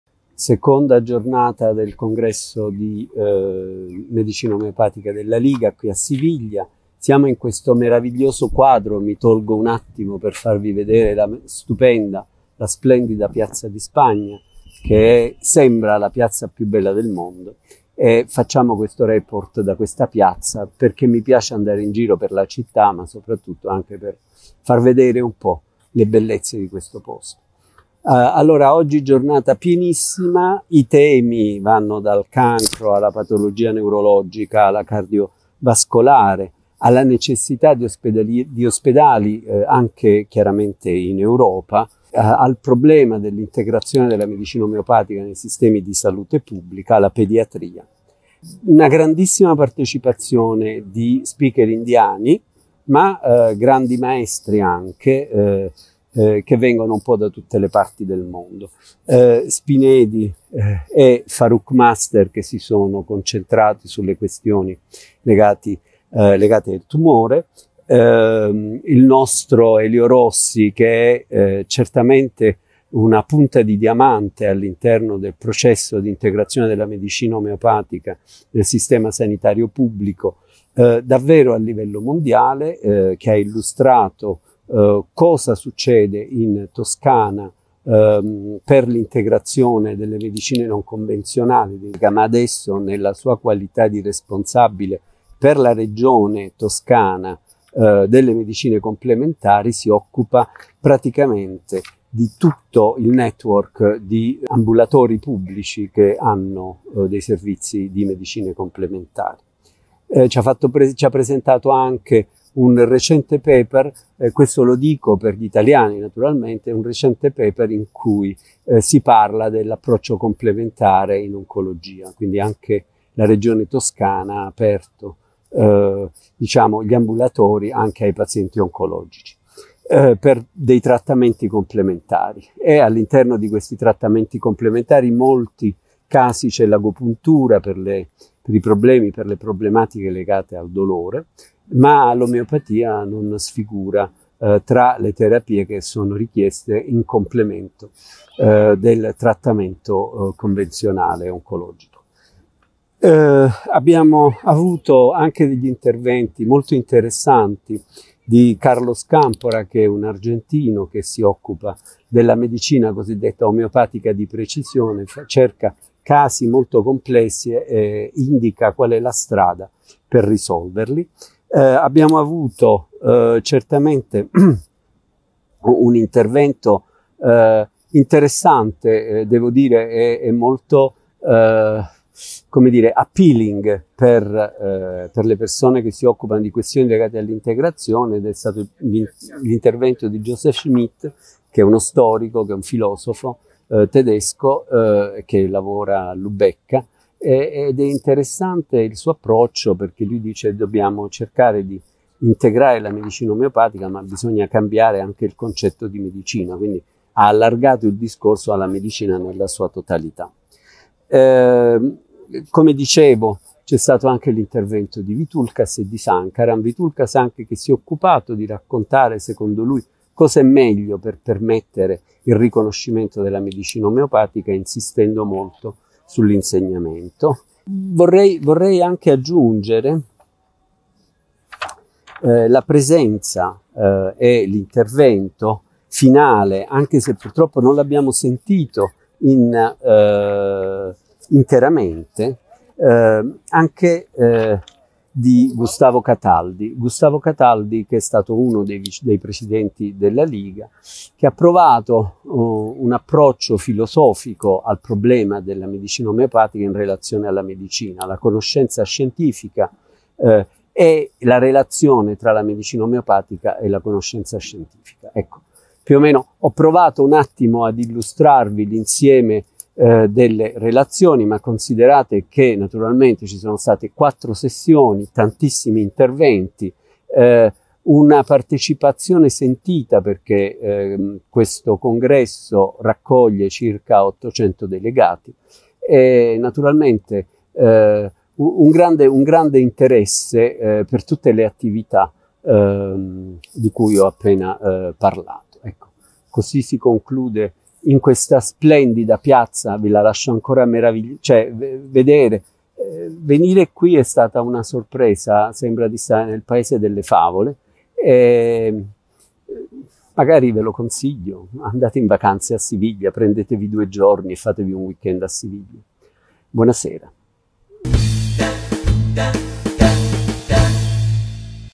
2° giorno - LMHI 2024 - 77° Congresso Mondiale di Omeopatia
Cronaca della seconda giornata di lavori, dal nostri inviato a Siviglia Dott. Vincenzo Rocco.